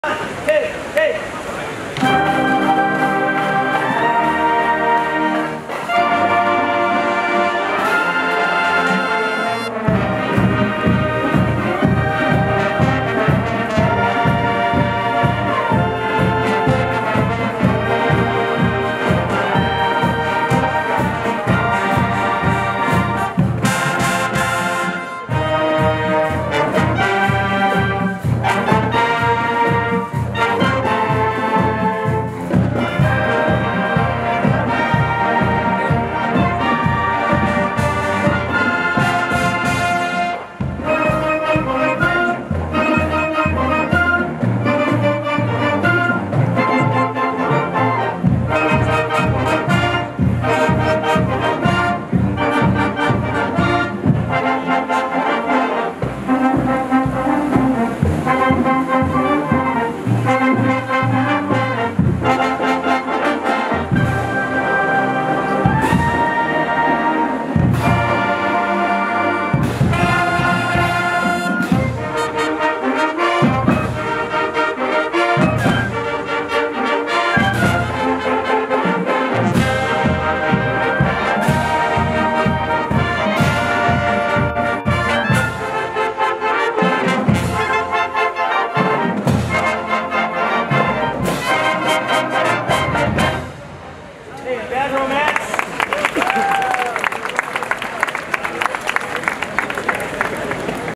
NAMM 2017 Opening Marching Band filmed by Jay Dexter
NAMM 2017 OPENING CEREMONY ANAHEIM'S SAVANNA MARCHING BAND (filmed by Jay Dexter)